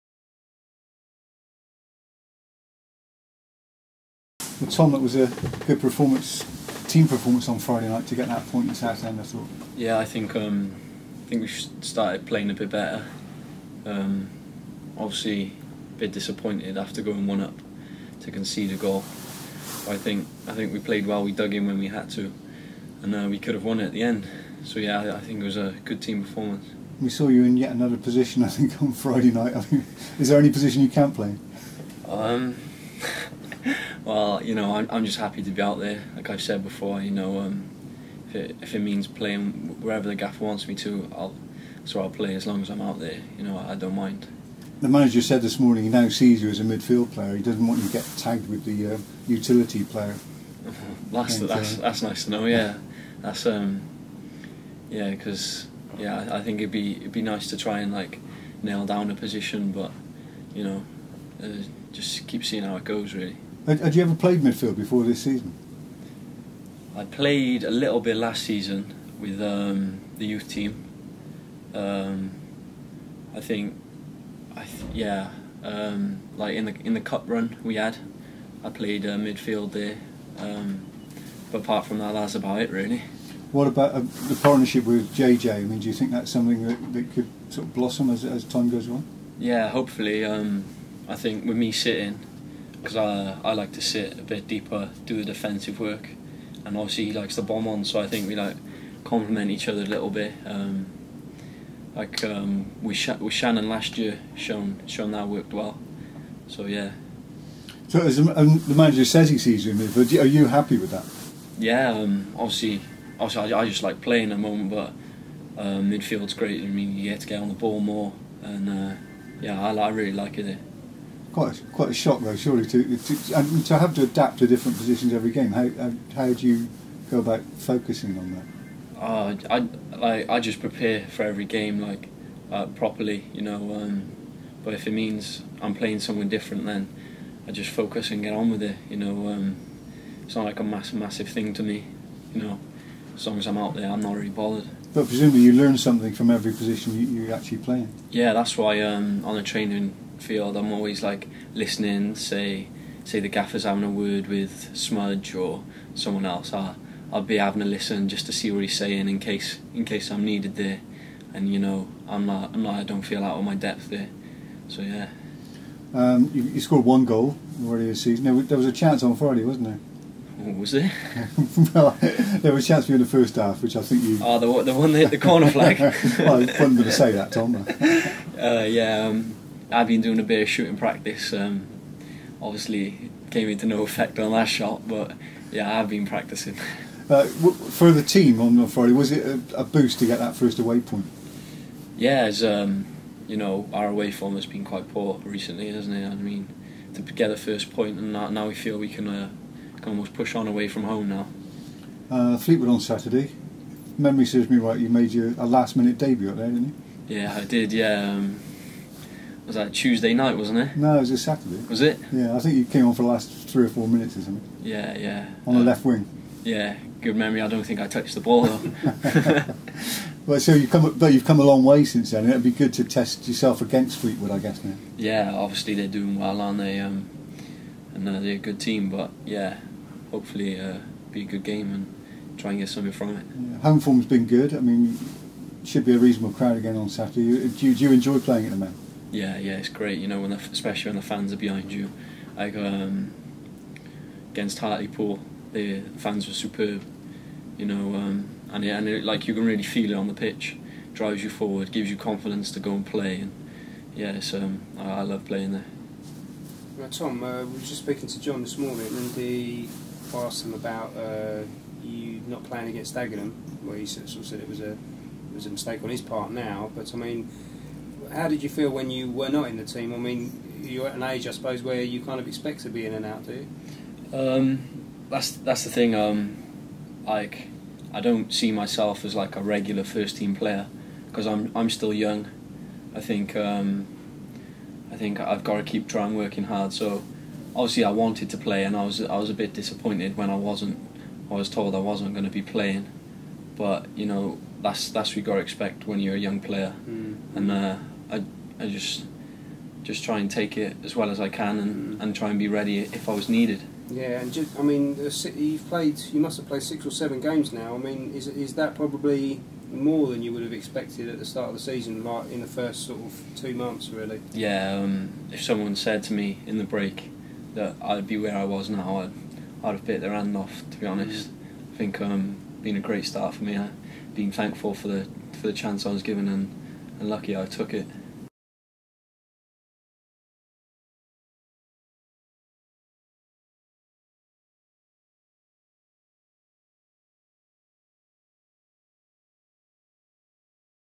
Bristol Rovers defender Tom Lockyer on trying to nail down a position.
Defender Tom Lockyer wants to make a position his own, after impressing in various positions across the park so far this season.